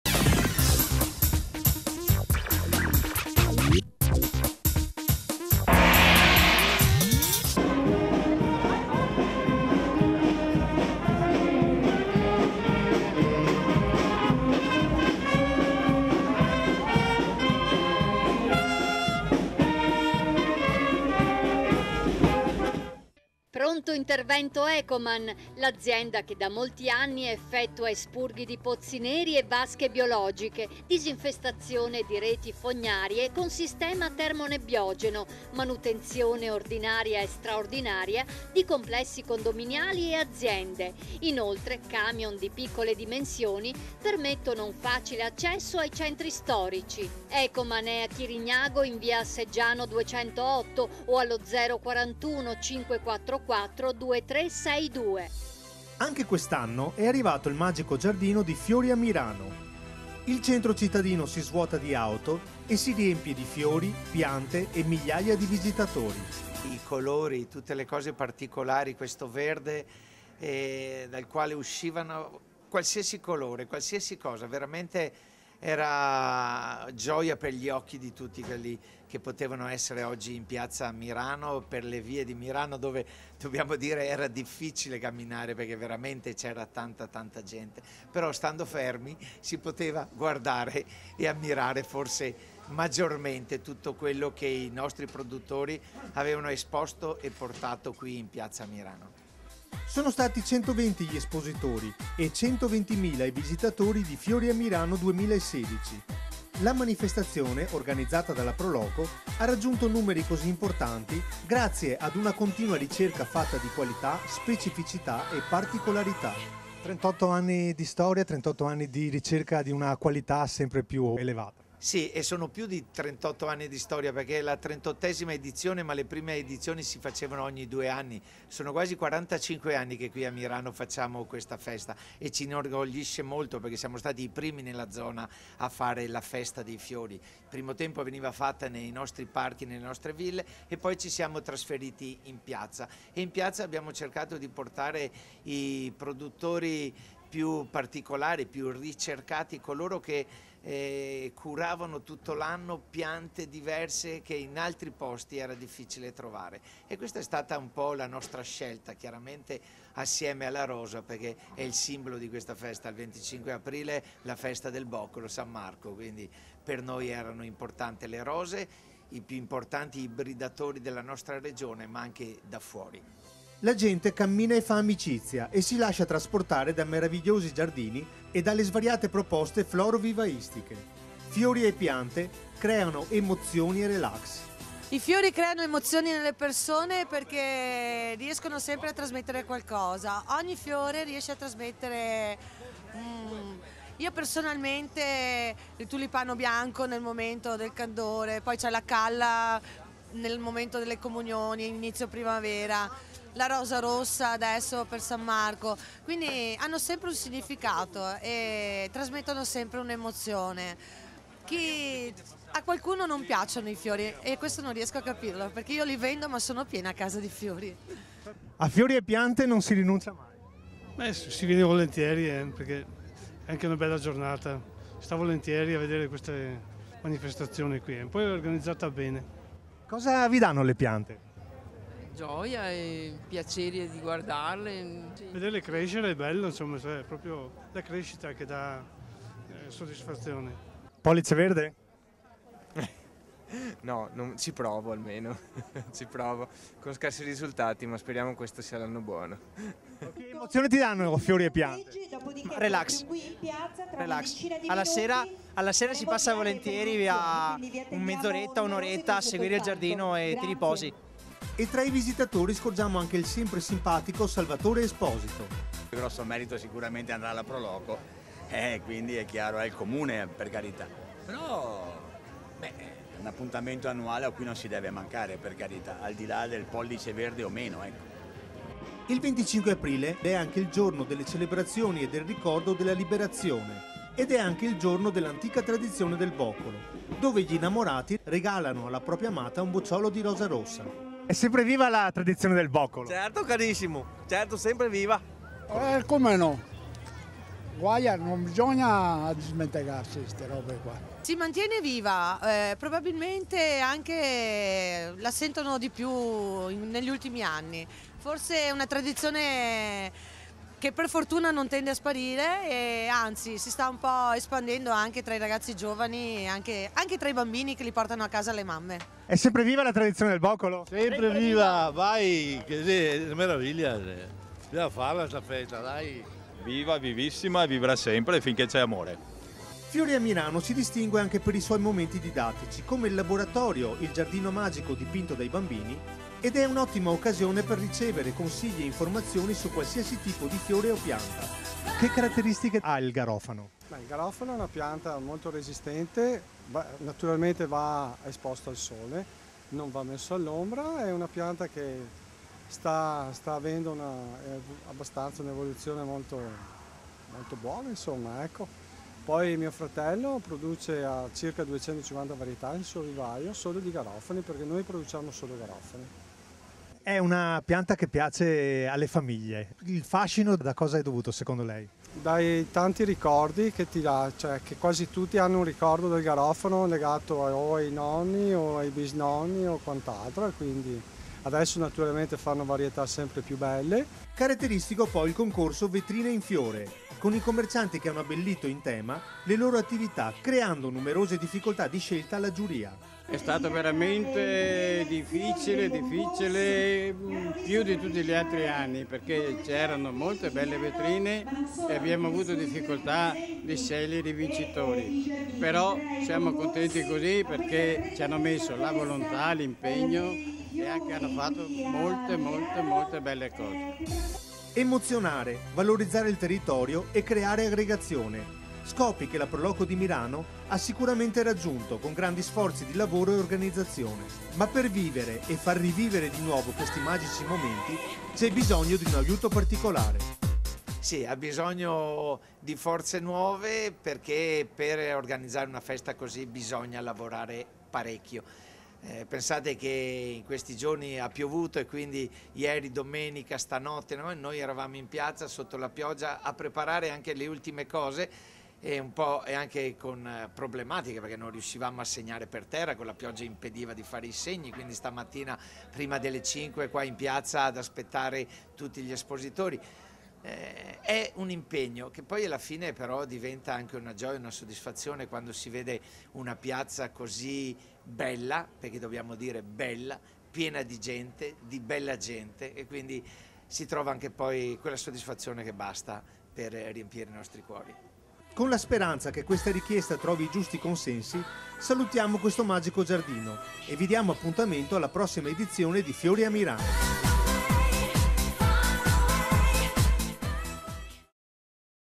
Fiori a Mirano 2016, un bagno di folla
Sono stati crca 120.000 i visitatori di "Fiori a Mirano" la manifestazione, organizzata dalla Pro Loco, che ha trasformato il centro cittadino in un magico giardino. Come ogni anno si ricorda il giorno della liberazione e si festeggiano fiori e piante. E' il giorno anche dell'antica tradizione del bocolo. Una giornata davvero speciale che merita di essere vissuta.